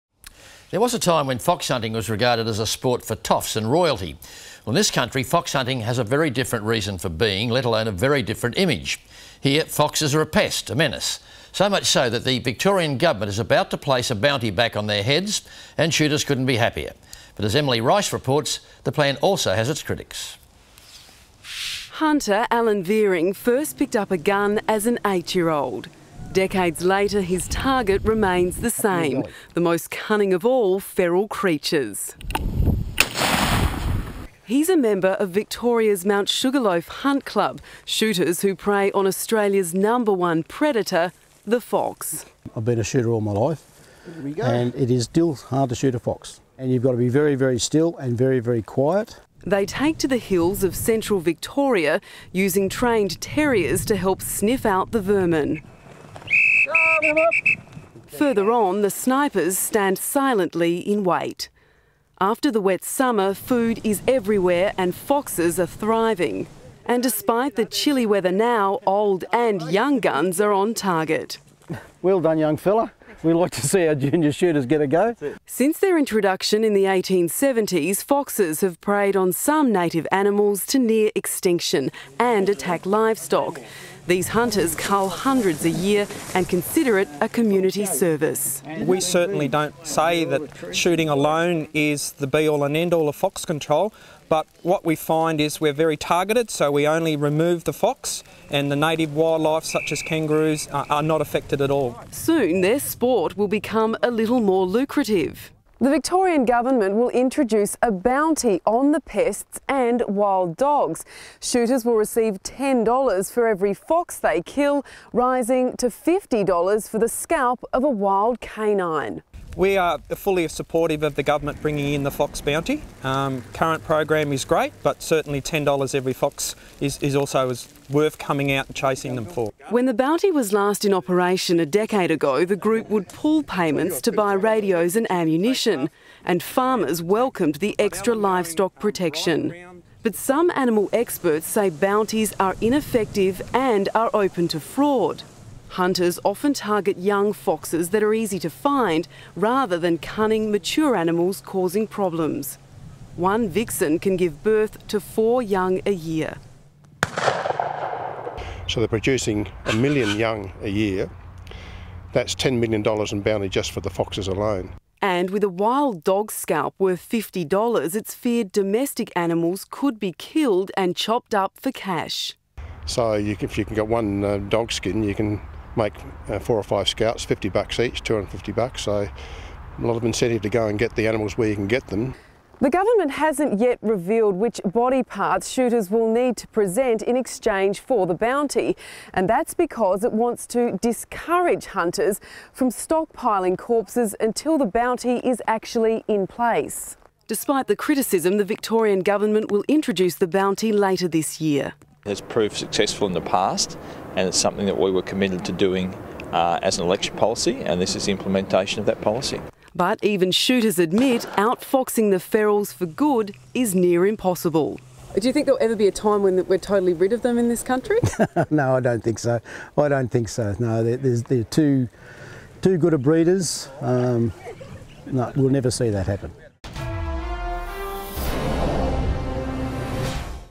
Bounty for foxes
Shooters to receive $10 for every fox they kill, under a new govt arrangement in Victoria.